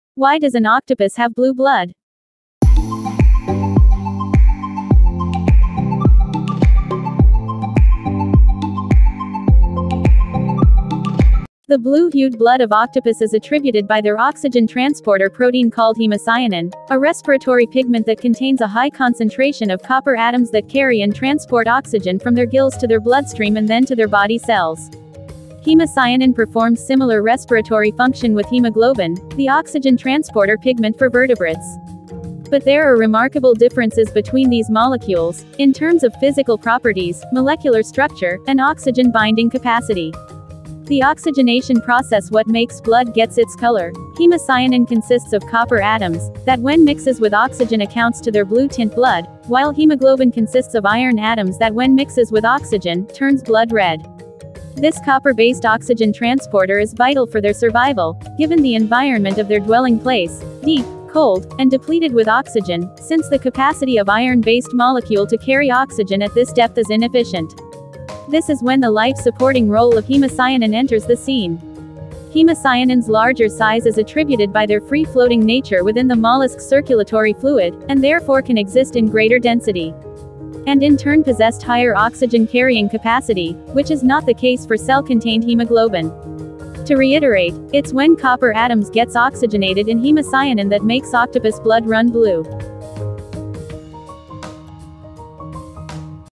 Why Does an Octopus Have Blue Blood?
The blue-hued blood of octopus is attributed by their oxygen-transporter protein, called hemocyanin, a respiratory pigment that contains a high concentration of copper atoms, that carry and transport oxygen from their gills to their bloodstream and then to their body cells.

Hemocyanin performs similar respiratory function with hemoglobin, the oxygen transporter pigment for vertebrates, but there are remarkable differences between these molecules, in terms of physical properties, molecular structure, and oxygen-binding capacity.

The oxygenation process what makes blood gets its color, hemocyanin consists of copper atoms, that when mixes with oxygen accounts to their blue tint blood, while  hemoglobin consists of iron atoms that when mixes with oxygen,  turns blood red.

This copper-based oxygen transporter is vital for their survival, given the environment of their dwelling place, deep, cold and depleted with oxygen, since the capacity of iron-based molecule to carry oxygen at this depth is inefficient, this is when the life-supporting role of hemocyanin enters the scene.

Hemocyanin's larger size is attributed by their free-floating nature within the mollusks' circulatory fluid (hemolymph), and therefore can exist in greater density , and in turn possessed higher oxygen-carrying capacity, which is not the
case for cell-contained hemoglobin.

To reiterate, it's when copper atoms gets oxygenated in hemocyanin that makes octopus' blood run blue.